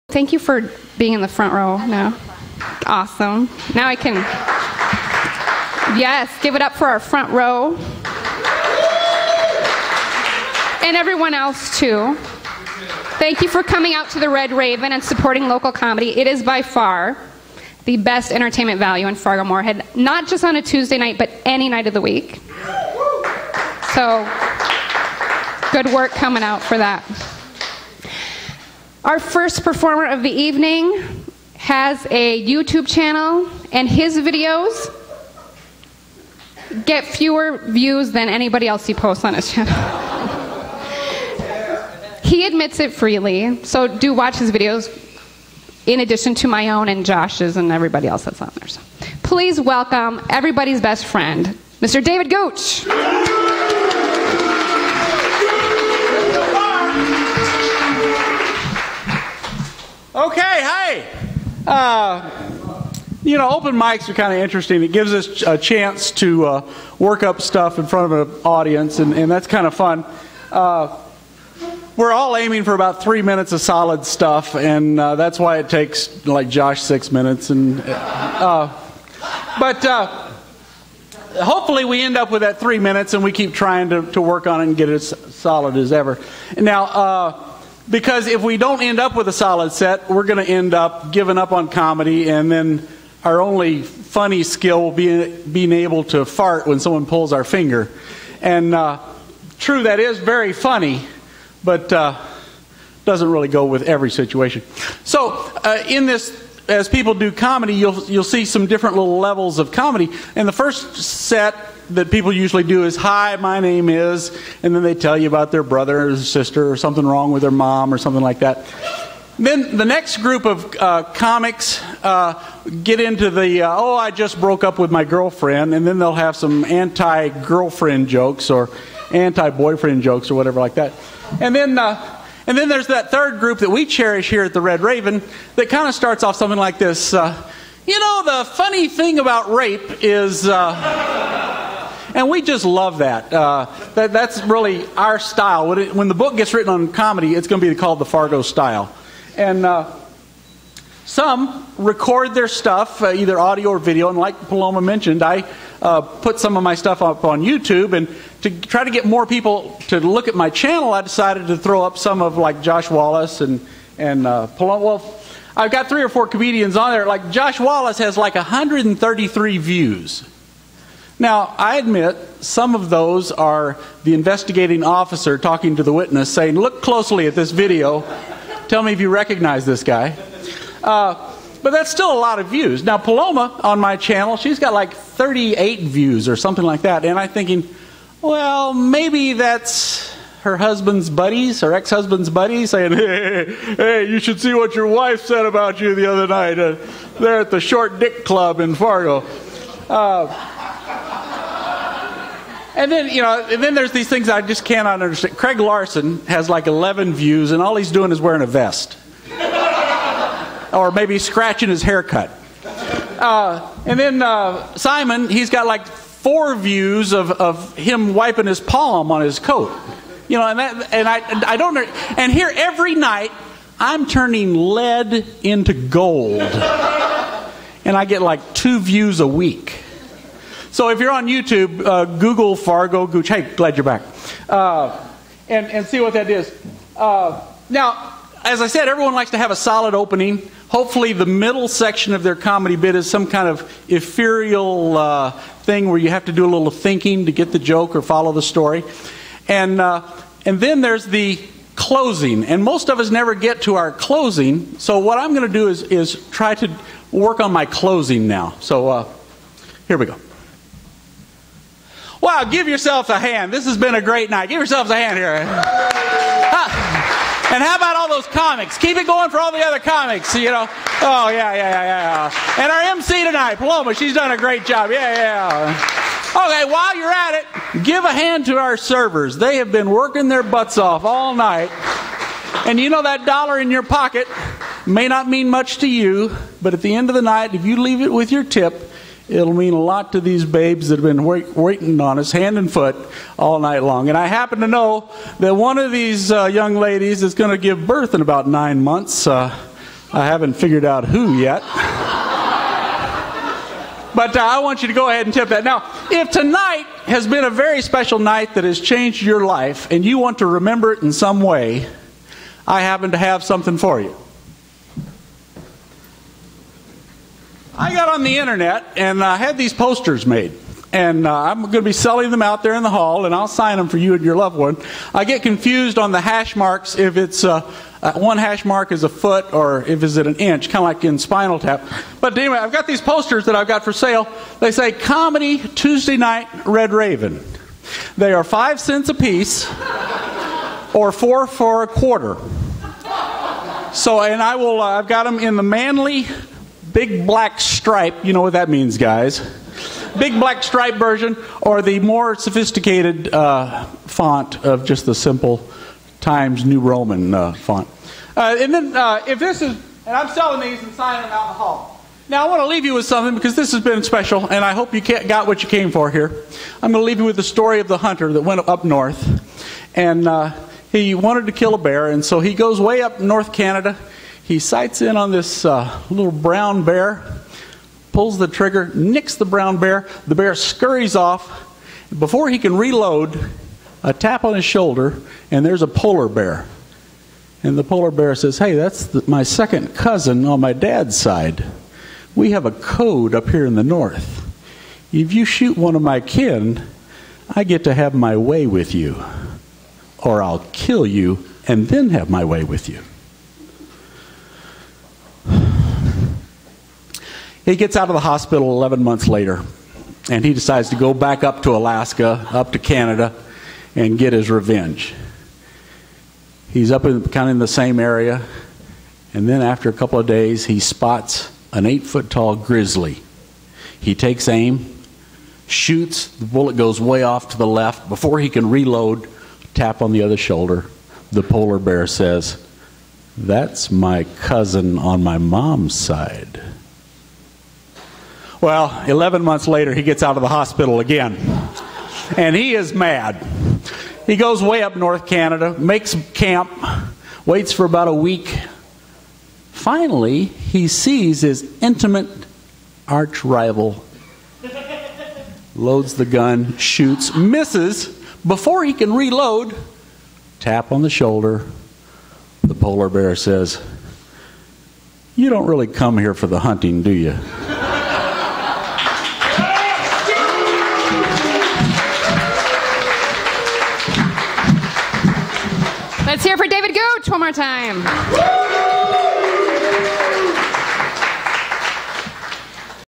in conculsion + bear hunt 6 7 2011.MP4
At an Open-Mic most work on their bit that will get them on tv someday. Sure-fire rock-em sock-em lead, they are trying to turn in to gold. In this bit I decide to work on my closing. ( tv is pretty much a long shot )